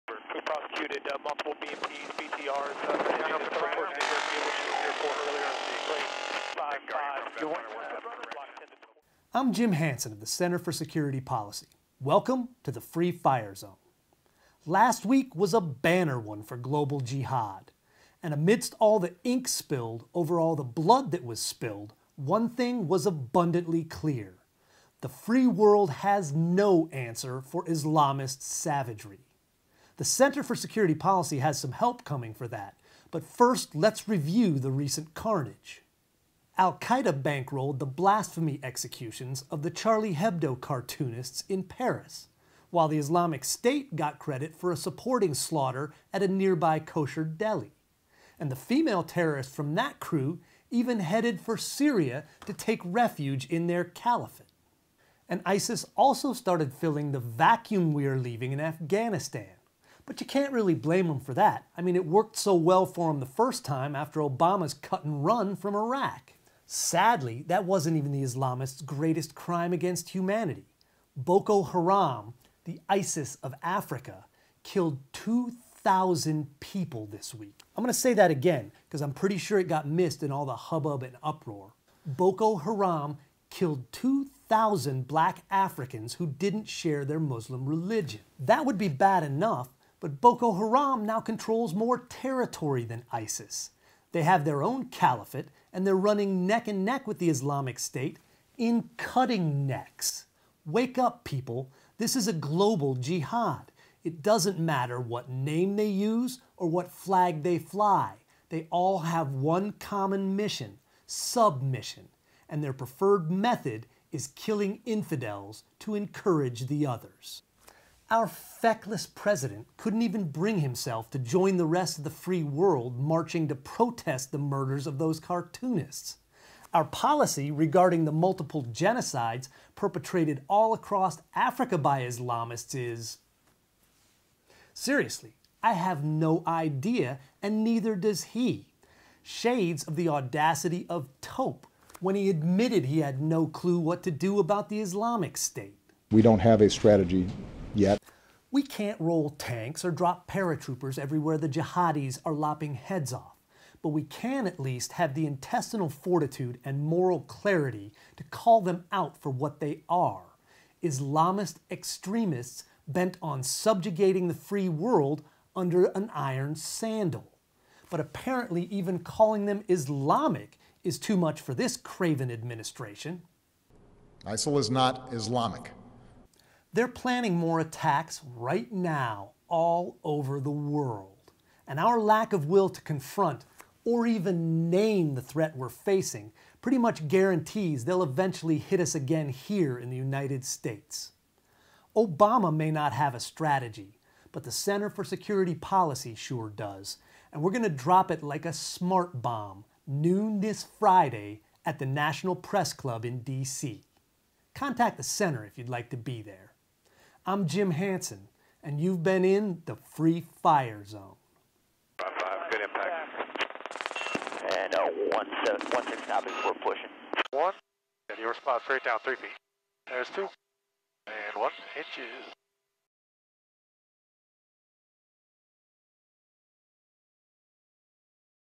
Free Fire Zone- A Strategy to defeat Global Jihad
Islamists are attacking all over the world. Obama may not have a strategy, but the Center for Security Policy does and we are releasing it at the National Press Club in DC. Noon Fri. Jan. 16th